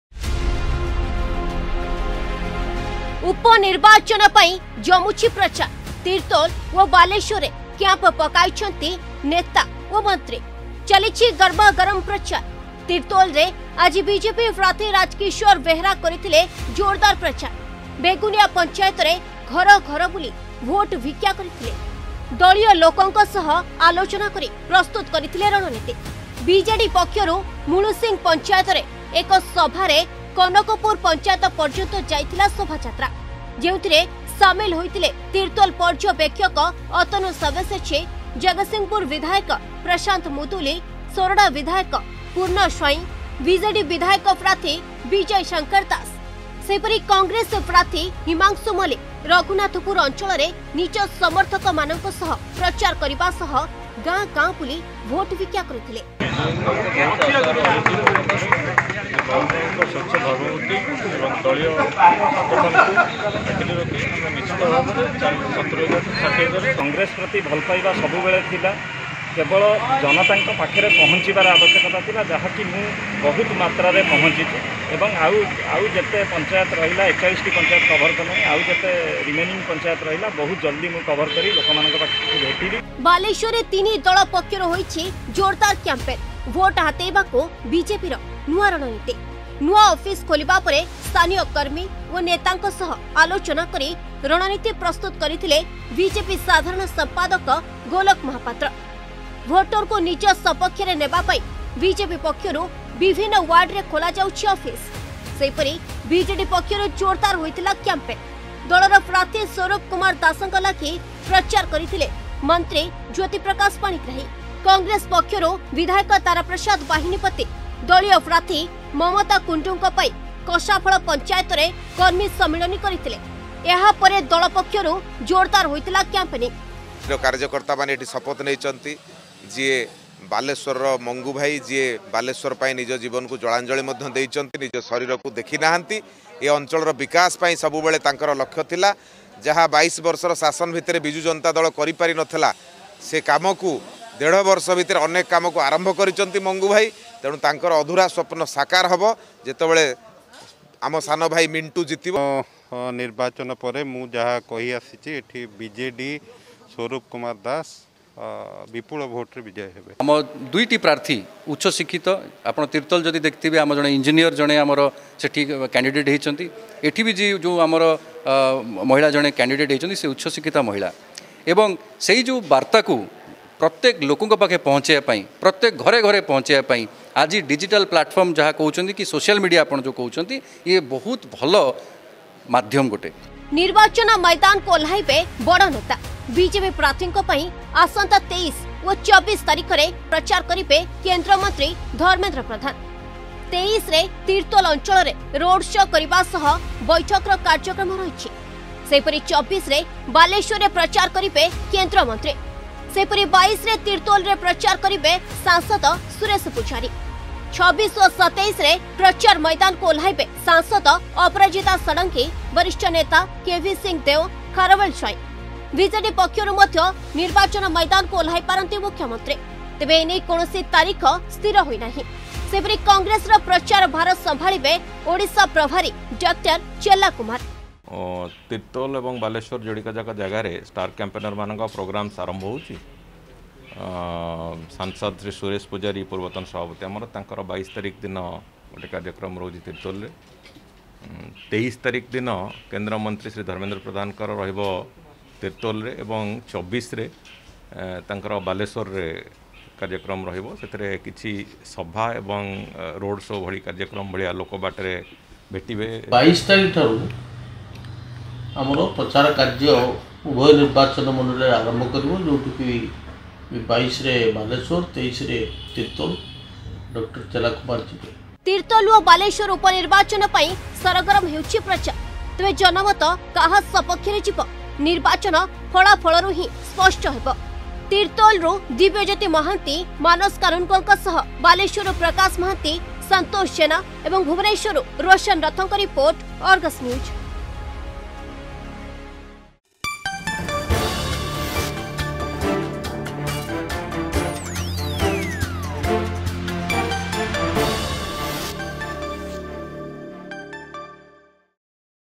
ଉପନିର୍ବାଚନ ପାଇଁ ଜମୁଛି ପ୍ରଚାର
ତିର୍ତ୍ତୋଲ ଓ ବାଲେଶ୍ବର ଉପନିର୍ବାଚନ ପାଇଁ ଜମୁଛି ପ୍ରଚାର । ମୈଦାନରେ ତିନି ଦଳର ପ୍ରାର୍ଥୀ ଭୋଟରଙ୍କୁ ମନାଇବାକୁ ପ୍ରାୟାସ କରୁଛନ୍ତି ।  ସେପଟେ ଆସନ୍ତା 23 ଓ 24 ତାରିଖରେ ବିଜେପି ପ୍ରାର୍ଥୀଙ୍କ ପାଇଁ ପ୍ରଚାର ମୈଦାନକୁ ଓହ୍ଲାଇବେ କେନ୍ଦ୍ରମନ୍ତ୍ରୀ ଧର୍ମେନ୍ଦ୍ର ପ୍ରଧାନ ...